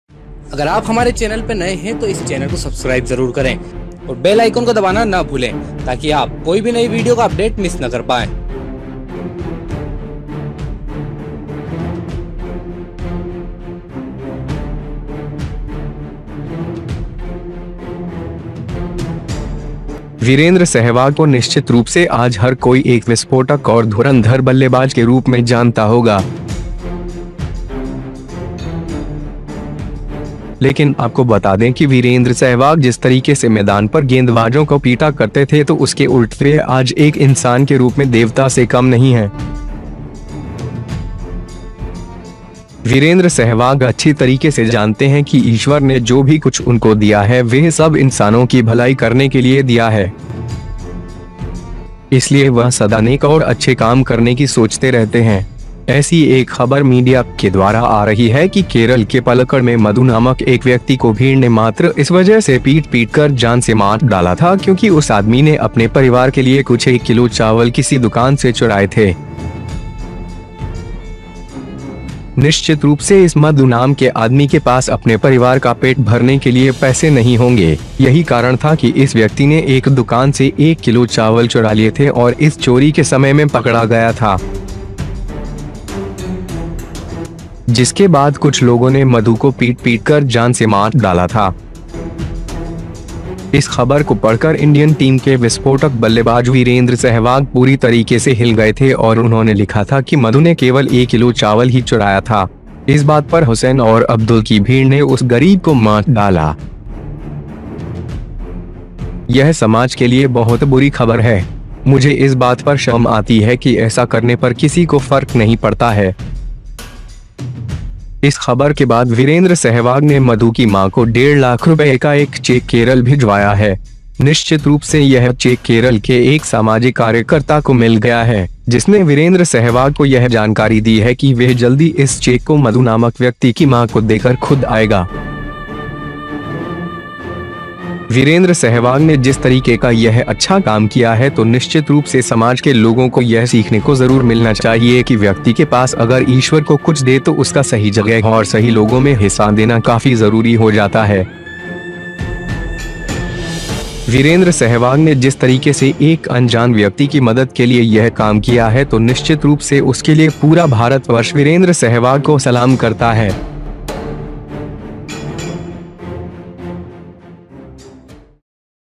वीरेंद्र सहवाग ने अपनी संपत्ति दान की
Indian Cricketer Virender Sehwag- वीरेंद्र सहवाग को निश्चित रूप से आज हर कोई एक विस्फोटक और धुरंधर बल्लेबाज के रूप में जानता होगा. लेकिन आपको बता दें कि वीरेंद्र सहवाग जिस तरीके से मैदान पर गेंदबाजों को पीटा करते थे तो उसके उल्ट वह आज एक इंसान के रूप में देवता से कम नहीं हैं.

वीरेंद्र सहवाग अच्छी तरीके से जानते हैं कि ईश्वर ने जो भी कुछ उनको दिया है वह सब इंसानों की भलाई करने के लिए दिया है. इसलिए वह सदा नेक और अच्छे काम करने की सोचते रहते हैं.